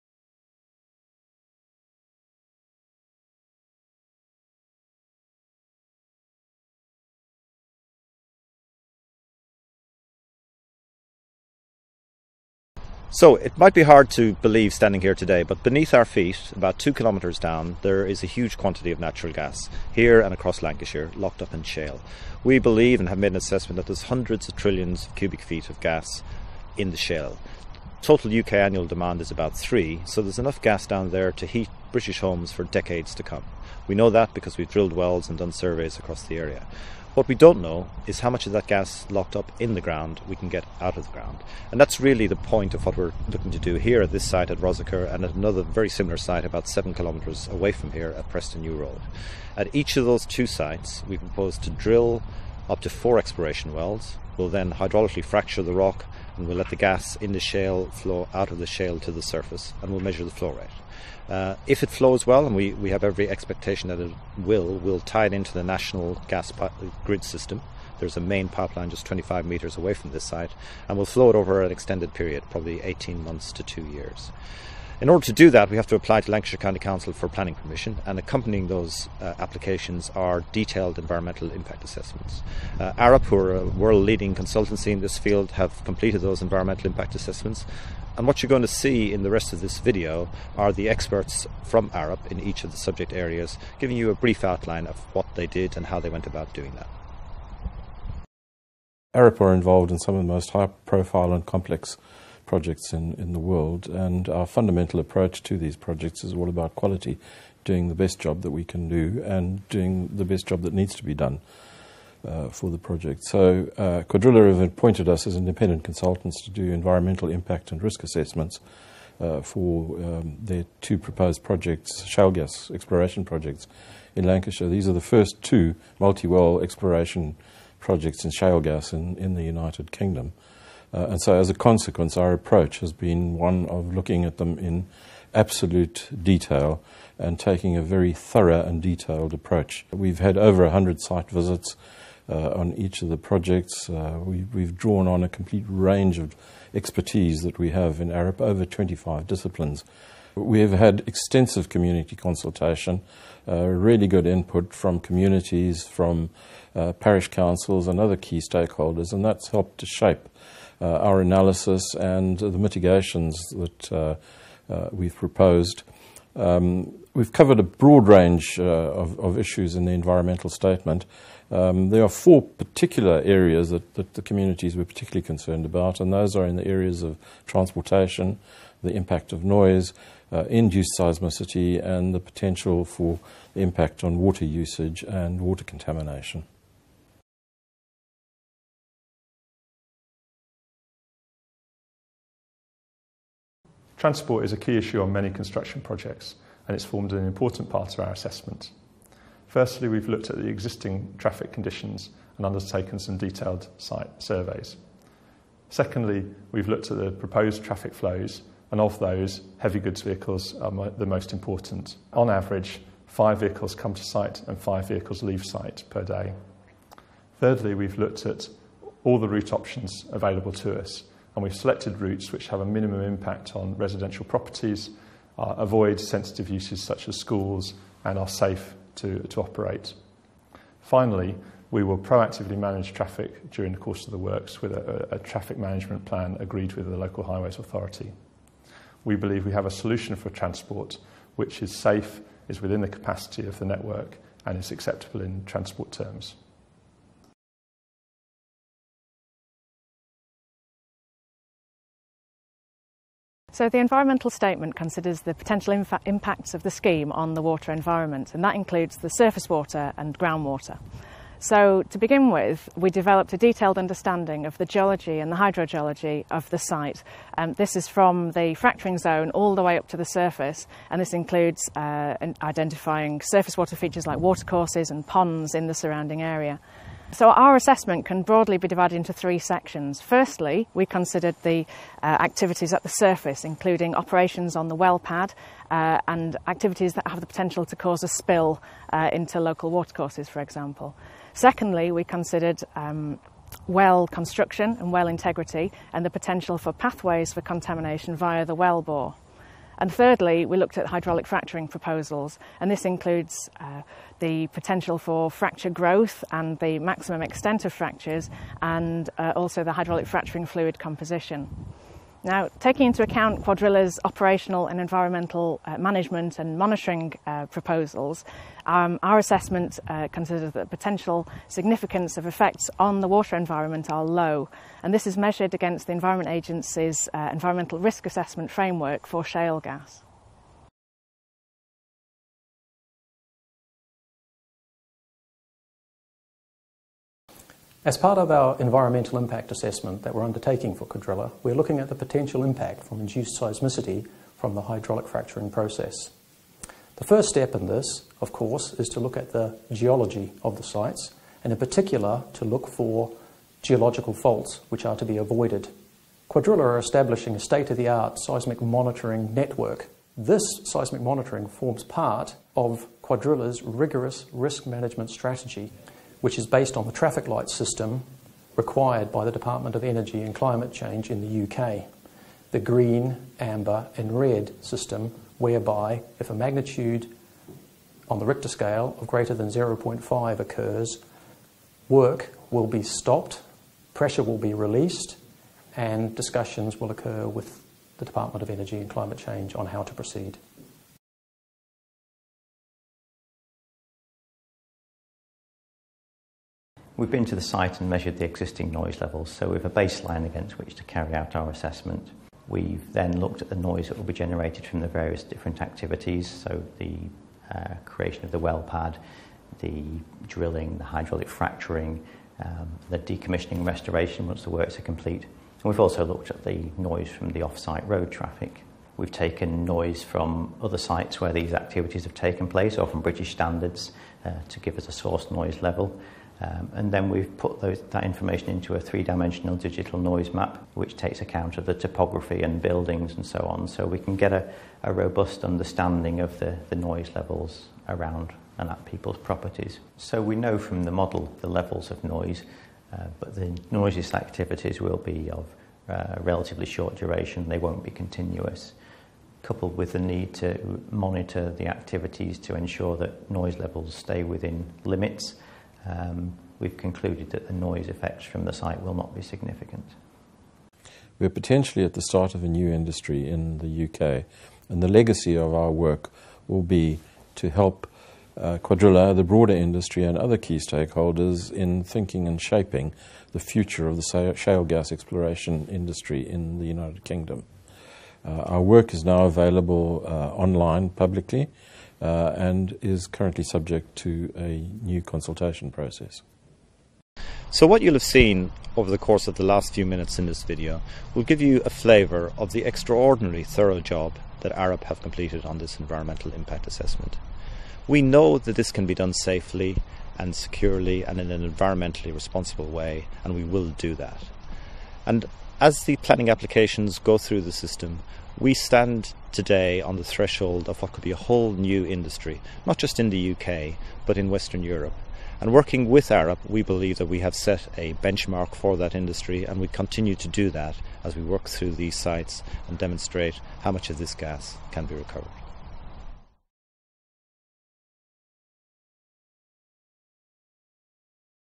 Cuadrilla Shale Gas
A video has been launched to provide an insight into the Environmental Impact Assessment (EIA) process and the resulting Environmental Statement (ES) that supports the planning applications for both of Cuadrilla’s proposed shale gas exploration sites in Lancashire.